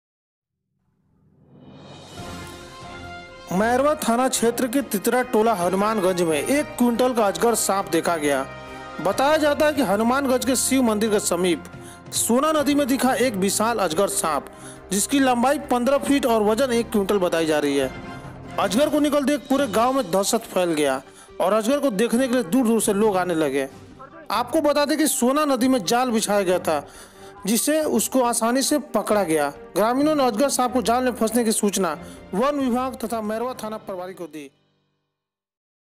15 फिट लम्बा और 1 क्विंटल वजन का अजगर निकलने से ग्रामीणों में फैला दहशत,मैरवा थाने के हनुमानगंज में
रिपोर्ट-आदित्य कुमार